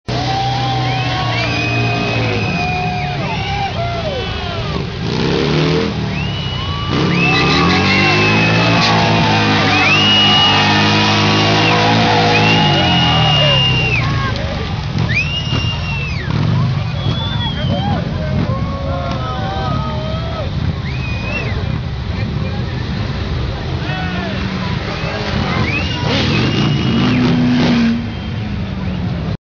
Harley Days Hamburg 2007 - Street Fun #5
Buells, Harleys and others having fun on the Reeperbahn during traffic.